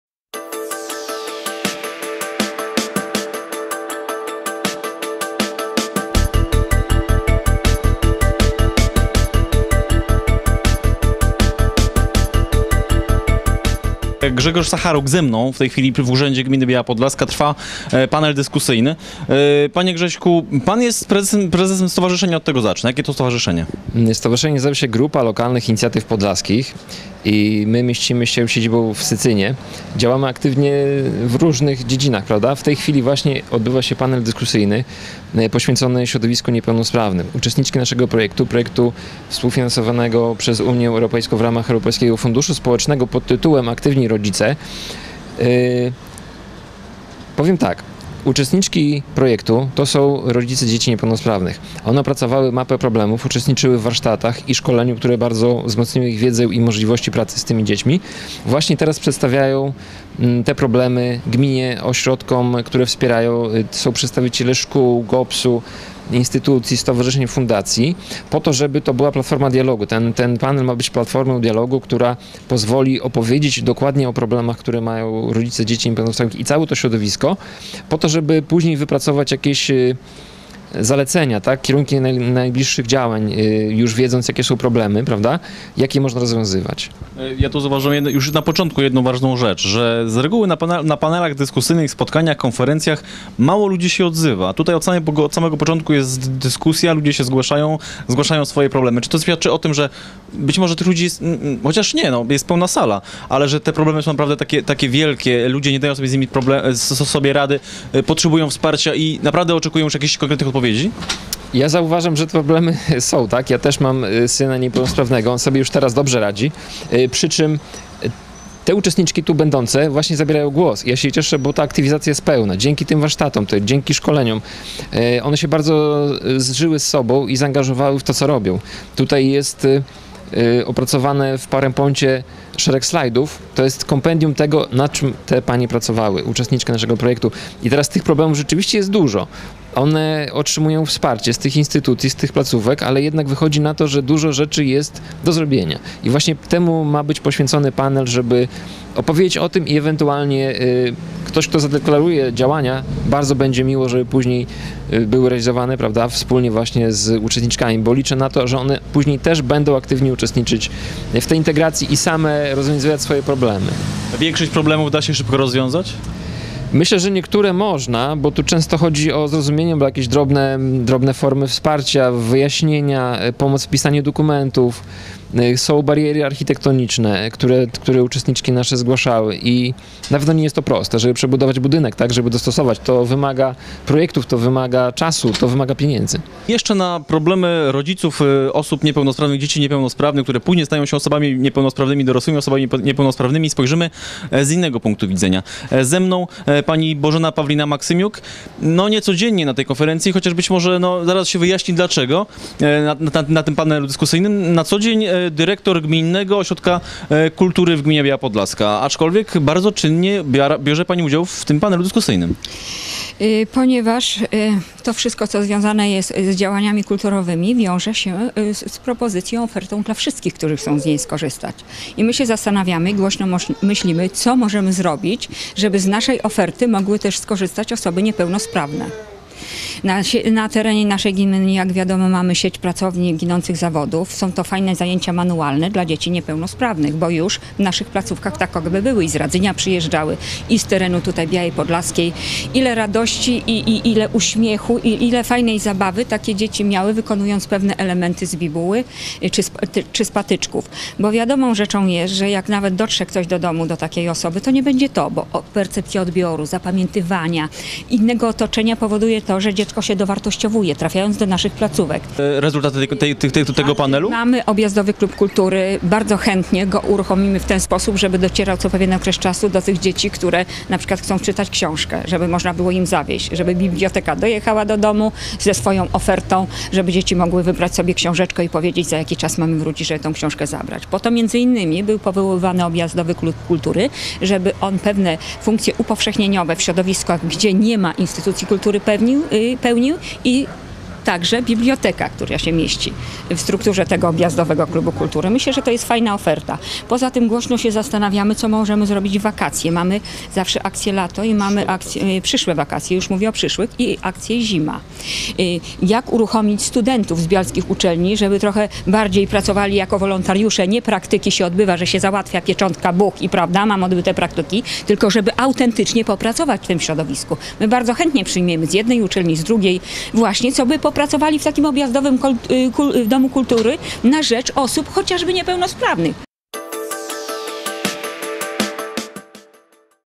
Panel dyskusyjny
„Aktywni Rodzice" to projekt realizowany m.in. przez Gminę Biała Podlaska. Czego on dotyczy i  w jaki sposób jest realizowany- wyjaśniano podczas specjalnego panelu dyskusyjnego podczas którego przedstawiono też mapę problemów osób niepełnosprawnych